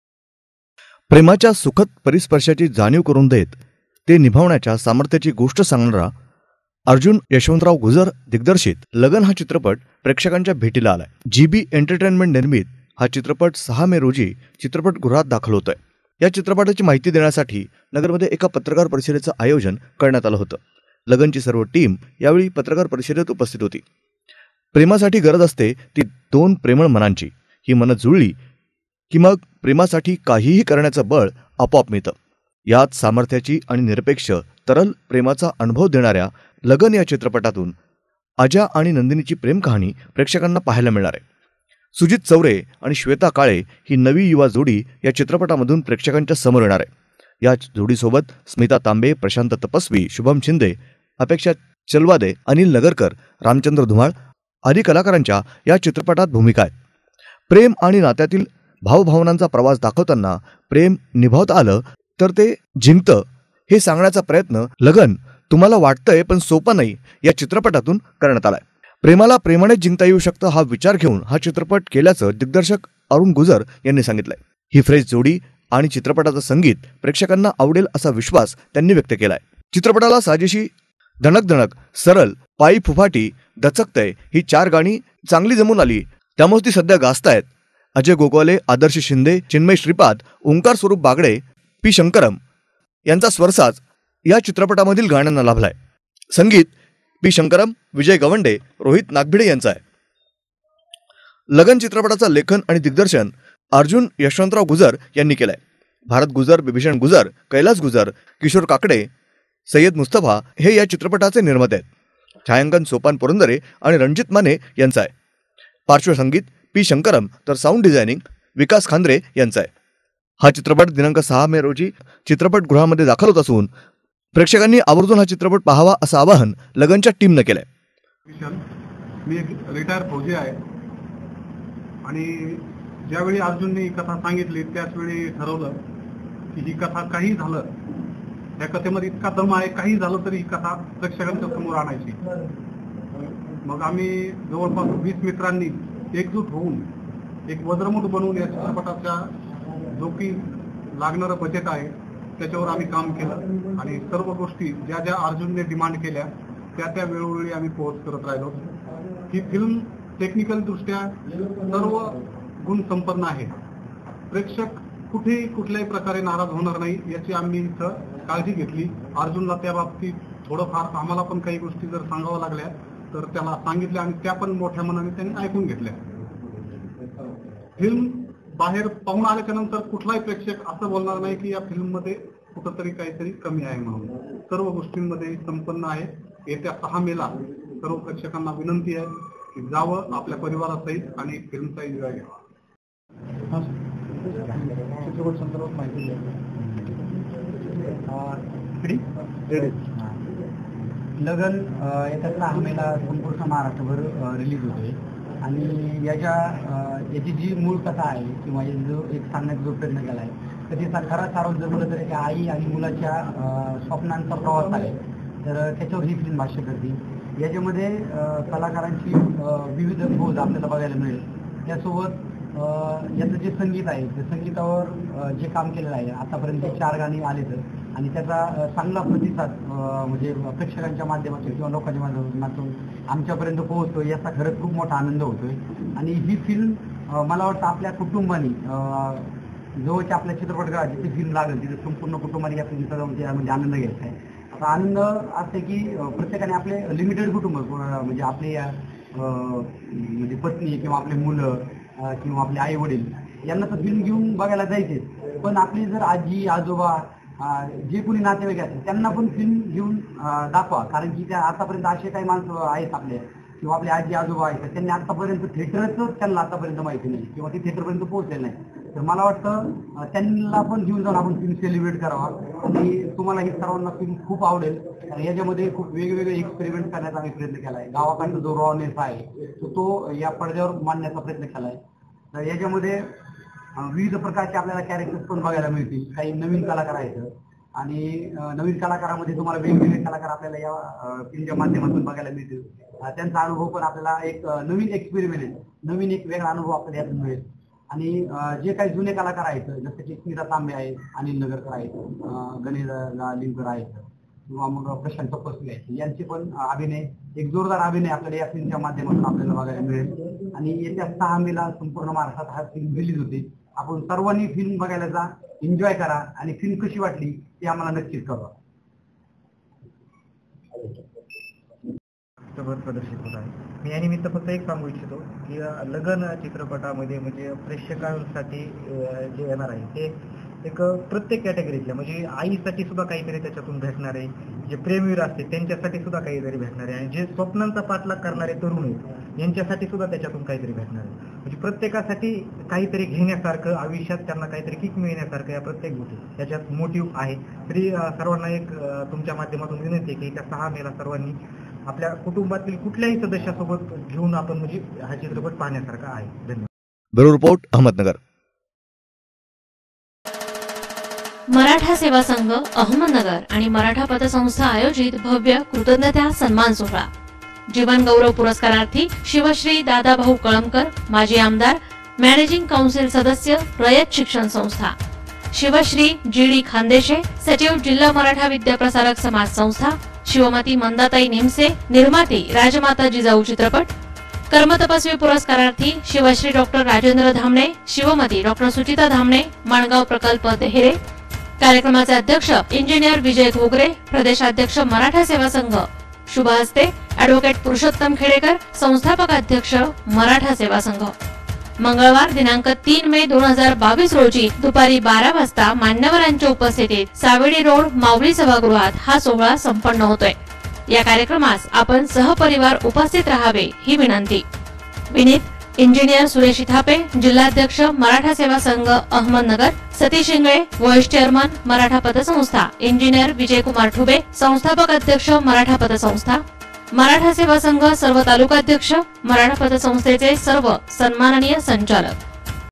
लगण हा चित्रपट सहा मे रोजी चित्रपट गृहात दाखल,
प्रेमाच्या सुखद परी स्पर्शाची जाणीव करून देत ते निभाना सामर्थ्याची गोष्ट सांगणं अर्जुन यशवंतराव लगण हा चित्रपट प्रेक्षकांच्या भेटीला आलाय चित्रपट सहा मे रोजी चित्रपट गृहात दाखल होत आहे या चित्रपटाची माहिती देण्यासाठी पत्रकार परिषदेचे आयोजन करण्यात आले होते यावेळी पत्रकार परिषदेत उपस्थित होते